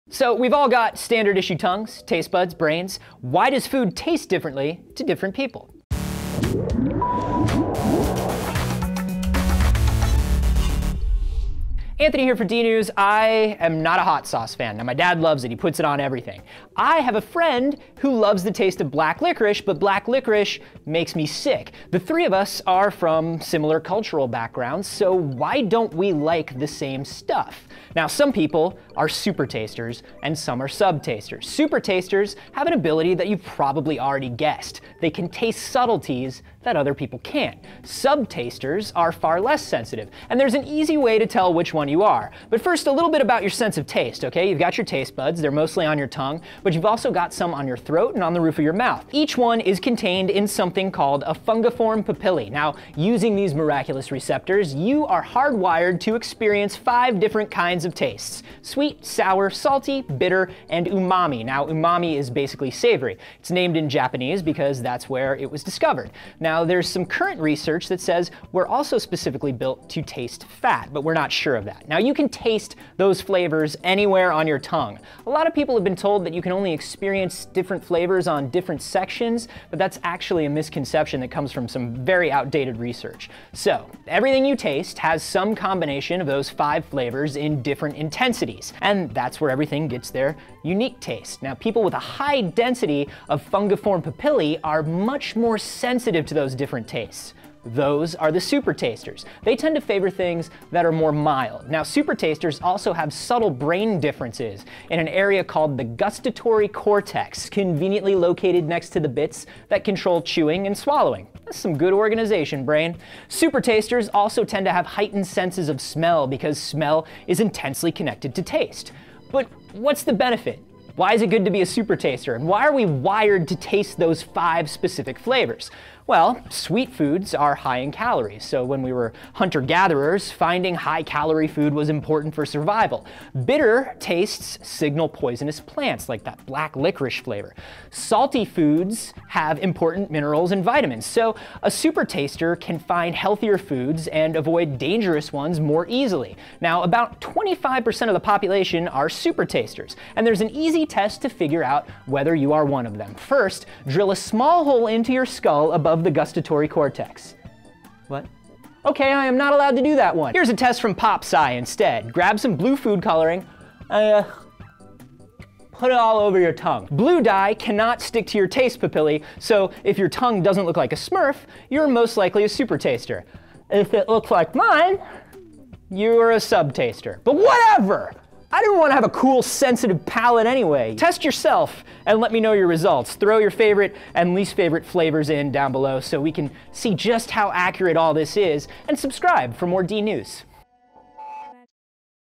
Why People Taste Things Differently
Everyone has different preferences when it comes to food, but why is that? Why the variation? As Anthony tells us, it's all down to something called papillae.

FYI: Why Does Some Food Taste Bad To Some People And Good To Others?
"People who have a lot of papillae—the bumps on our tongue, most of which house our taste buds—often find flavors overwhelming."

"Until recently, scientists have accepted four basic tastes. You know them well -- sweet, salty, sour and bitter."

How does our sense of taste work?
"A bitter pill, sour grapes or sweet nothings -- descriptions of taste are very often associated with strong emotions."

How Does the Sense of Taste Work?
"The sense of taste begins with the taste buds, located on top of the fungiform papillae, or the large bumps on the tongue."